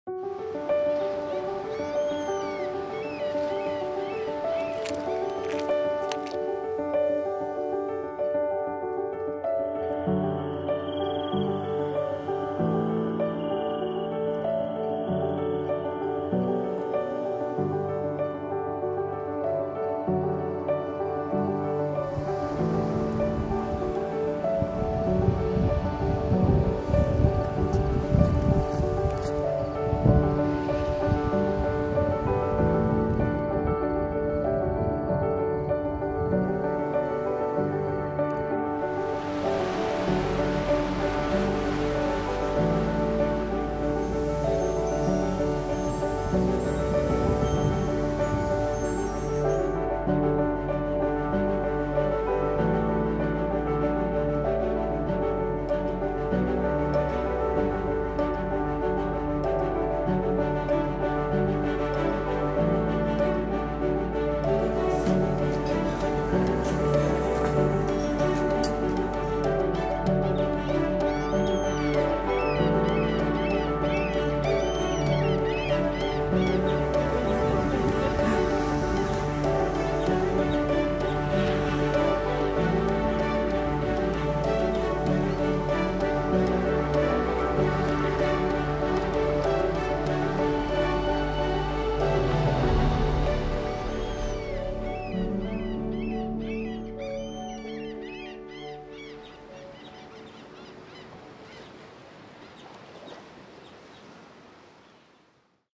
Exploring New England | Cape Cod
A travel video featuring charming Cape Cod, MA.